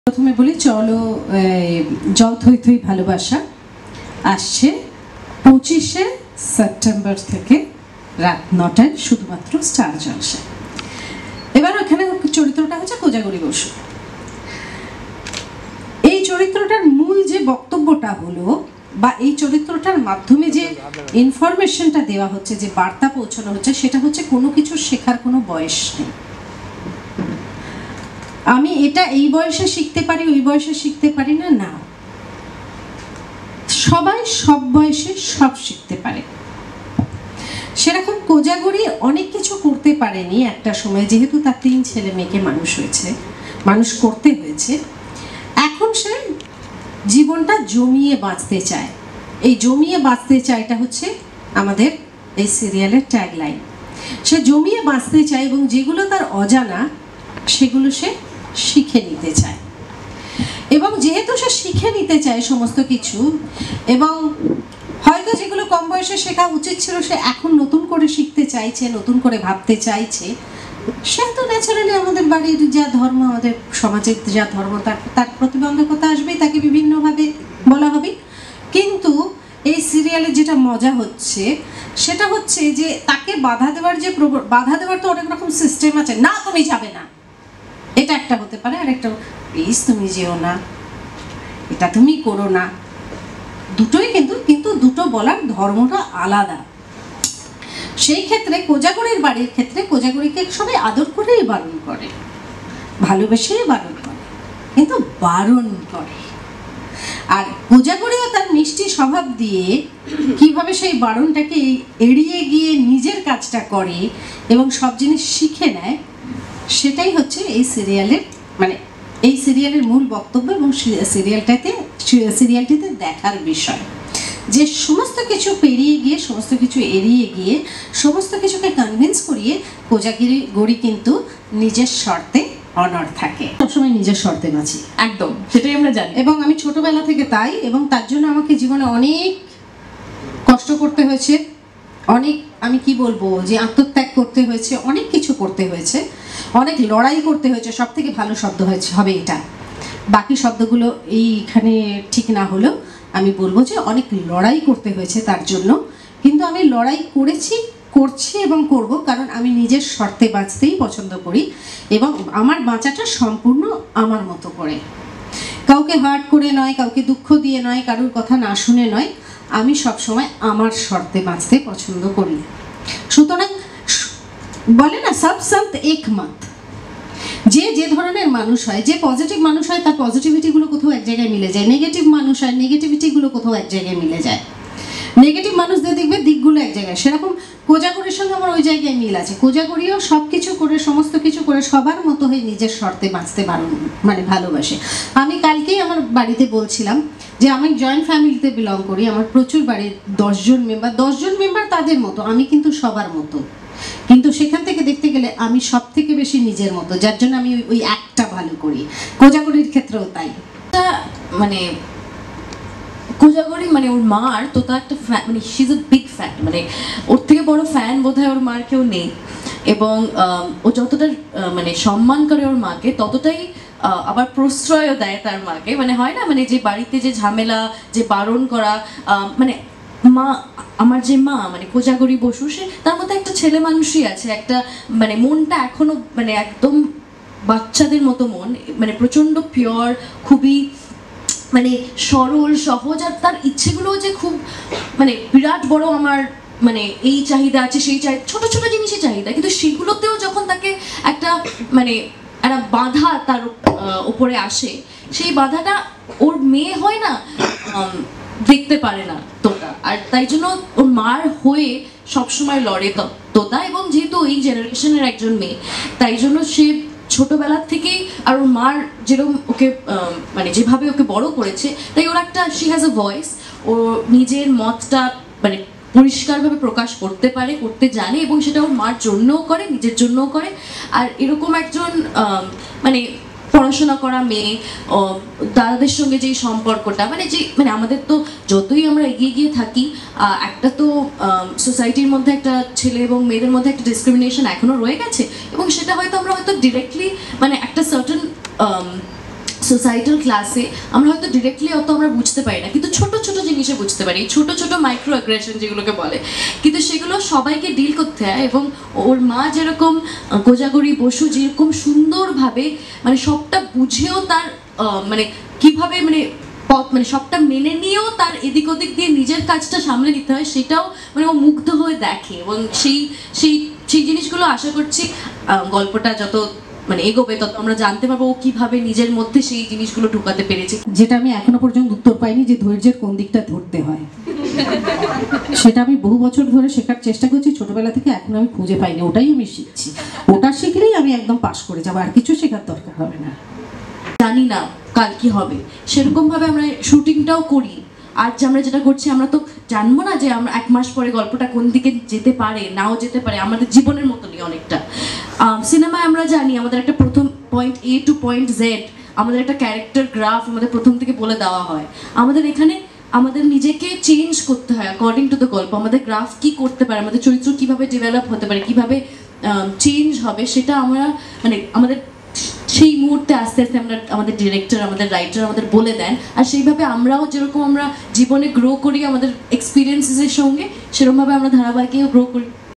"I Have Always Lived Life On My Own Terms"-Aparajita Adhya
The press meet of Jol Thoi Thoi Bhalobasha was graced by Aparajita Adhya who plays Kojagori and Anusha Viswanathan who plays the role of her daughter Tota.